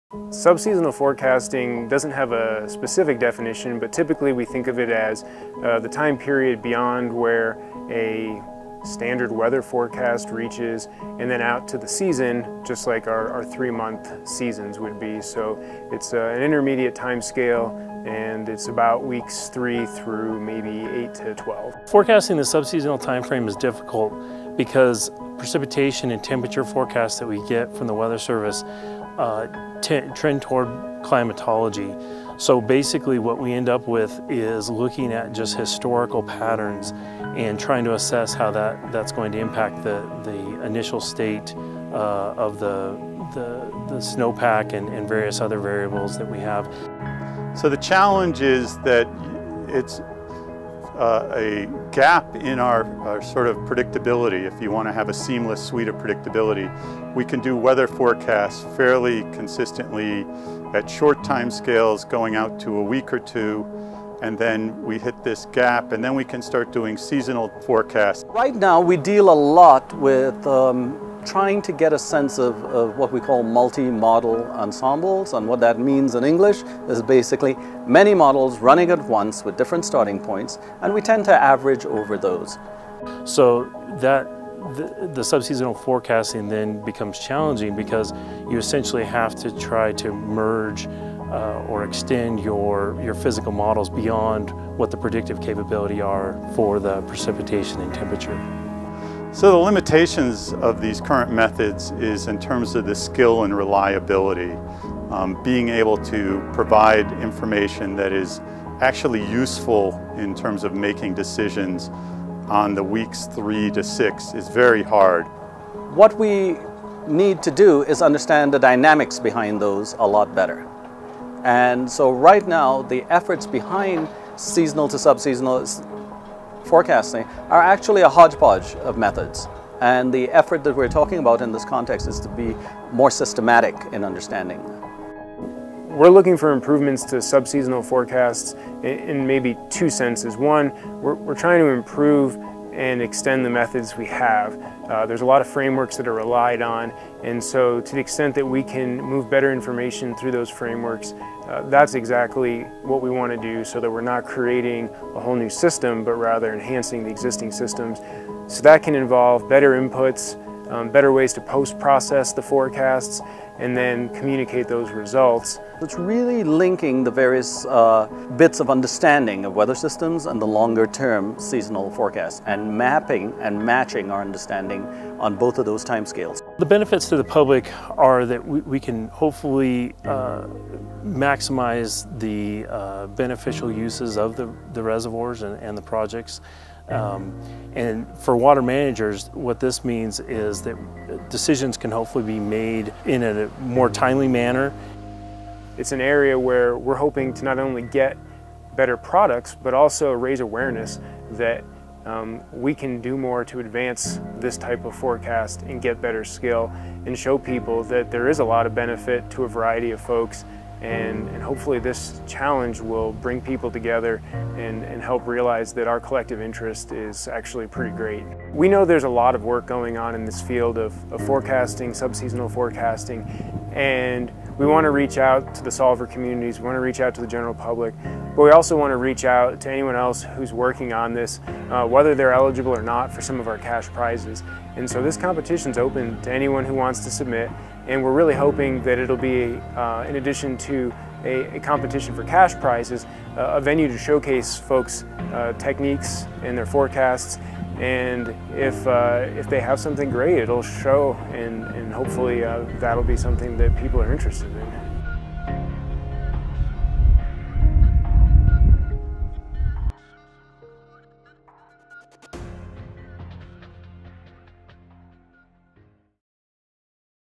Sub-Seasonal Climate Forecast Rodeo
Water managers need more skillful information on weather and climate conditions with lead-times ranging from 15 days to 45 days and beyond. Lacking skillful sub-seasonal information limits water managers’ ability prepare for shifts in hydrologic regimes, such as the onset of drought or occurrence of wet weather extremes. The challenge of sub-seasonal forecasting is that it encompasses the time frame where initial state information (e.g., coupled land atmosphere processes) becomes less important and slowly varying long term states (e.g., sea surface temperatures, soil moisture, snow pack) become more important to prediction skill.